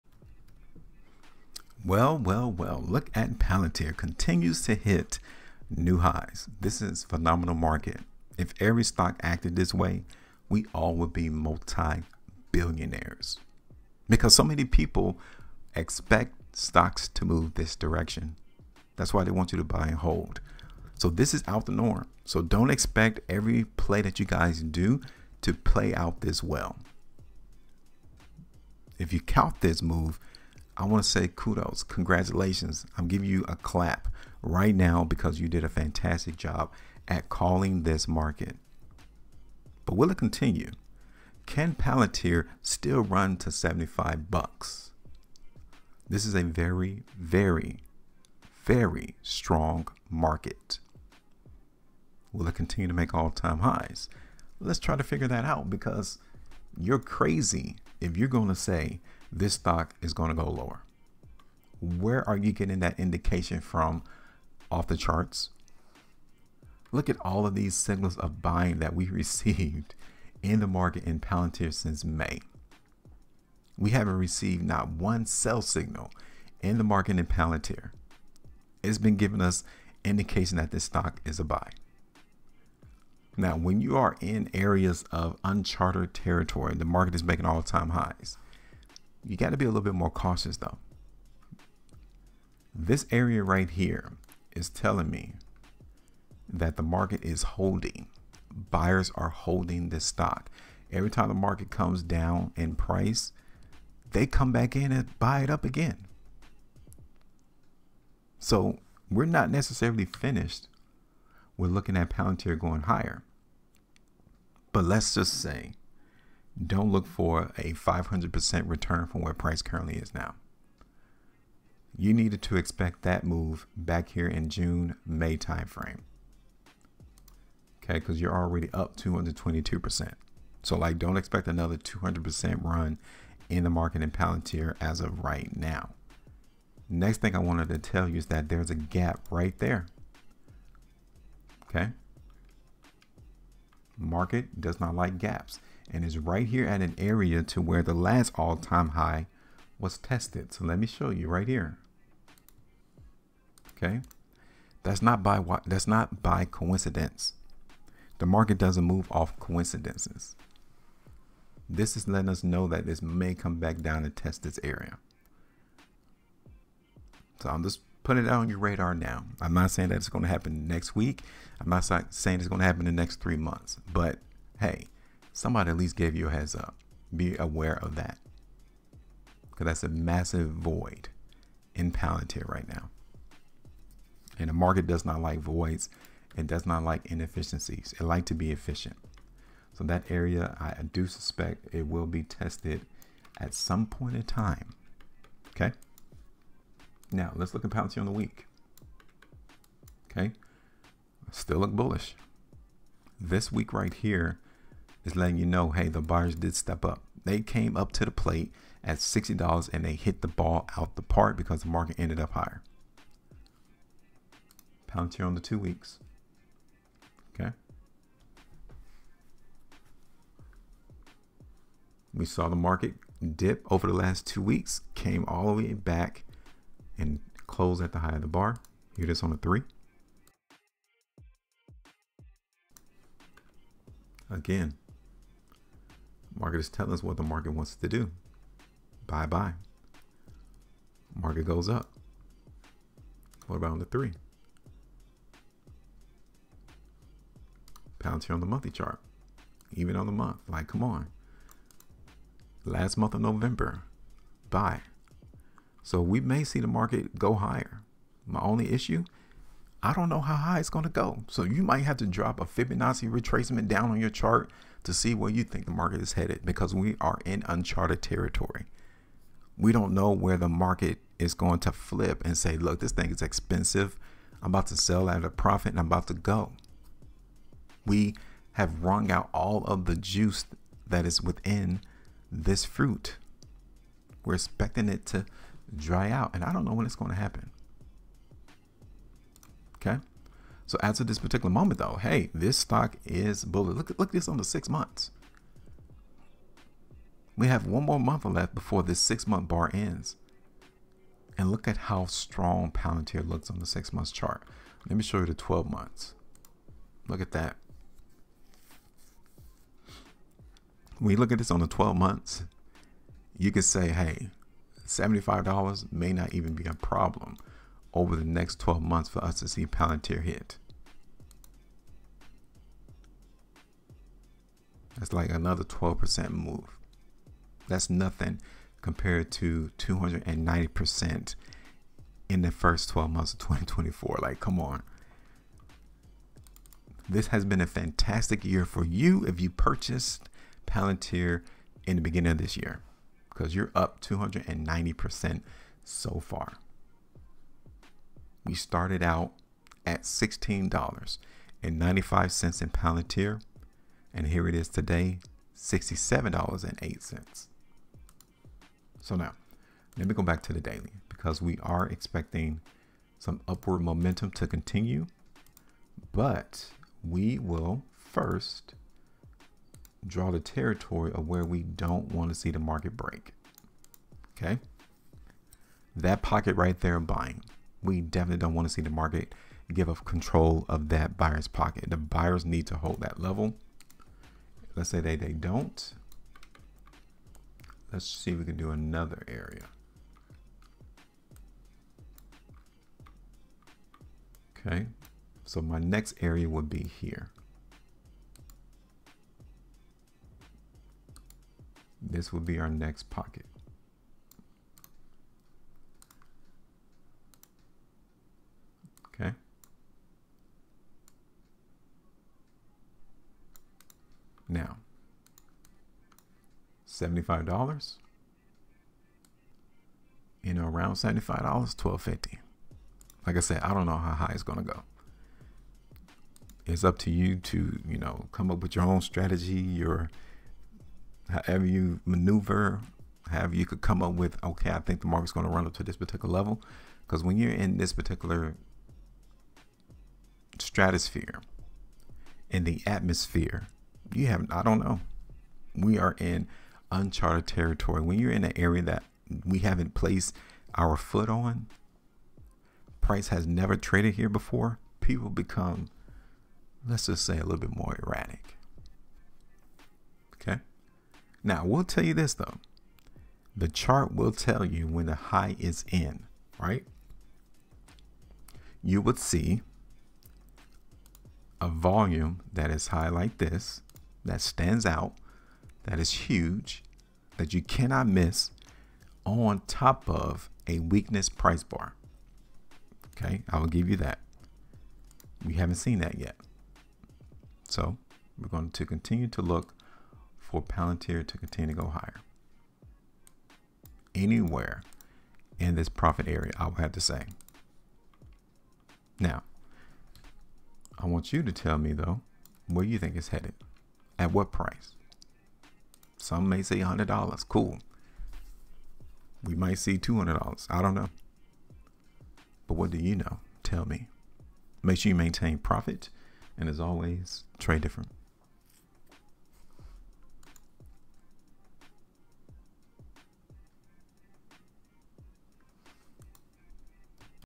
Palantir Stock Hit New Highs! Is $75 Next? PLTR Stock Prediction
Massive PLTR stock surge? Resistance ahead at $70? Palantir stock buyers crush sellers - how long will it last? PLTR stock bulls power through to all-time highs?